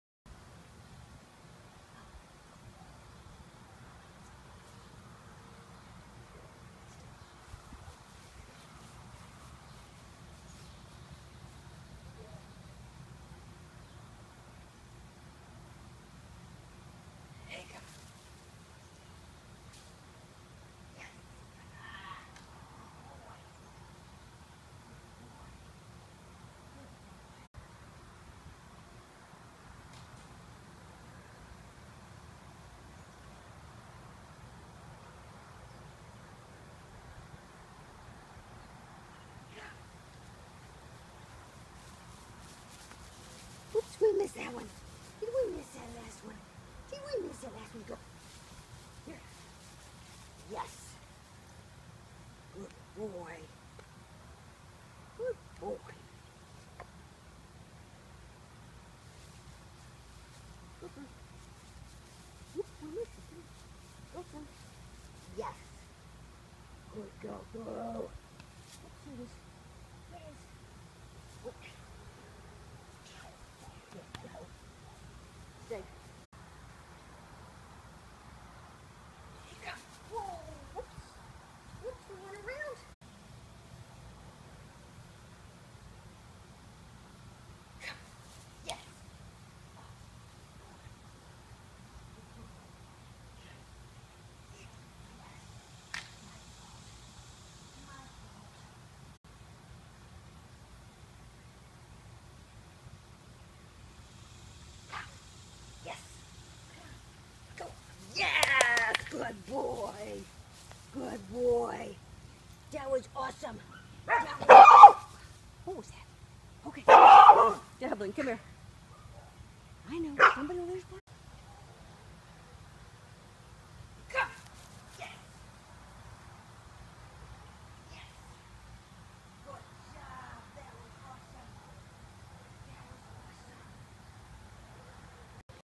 Come Jumps
Working on Dublin's  "restrained" recalls and jumps combined.